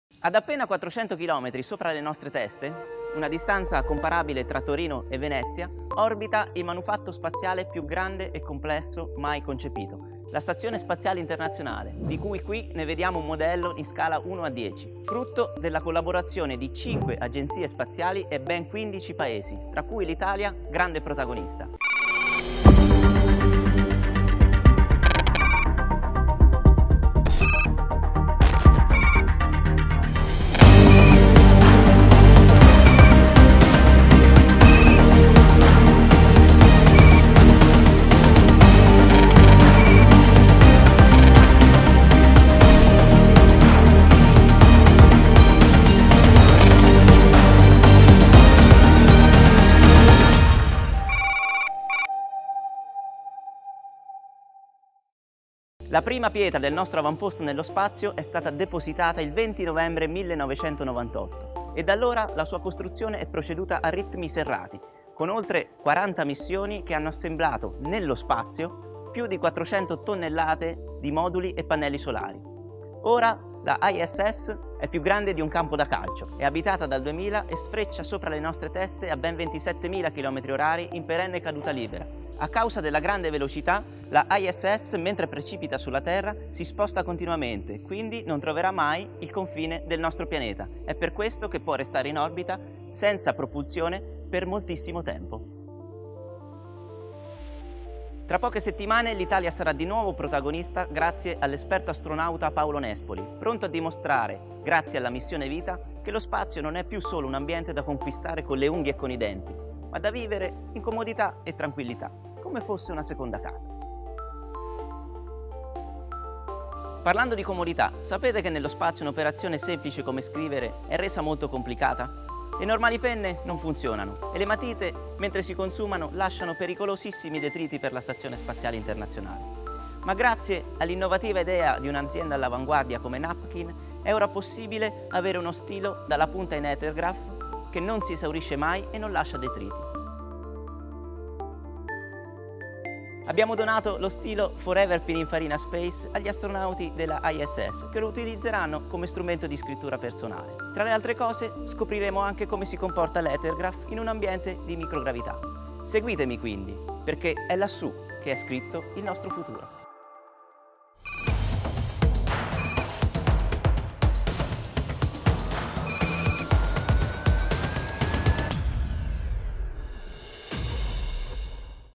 EPISODE 01 - Discovering the Space Mission
This year, 400 km from the earth, within the largest space structure ever built, 6 astronauts will complete one of the most important Science Missions ever.

NAPKIN® and Pininfarina donated to the crew members a special writing tool, ForeverPininfarinaSpace, the latest innovation created by the collaboration between the two brands, which will accompany them during the whole mission.

🎞️ Here is the first of the 15 episodes of the fantastic story that you can follow exclusively on social channels NapkinForever and Pininfarina.

Follow us and interact with us, because space is listening to you!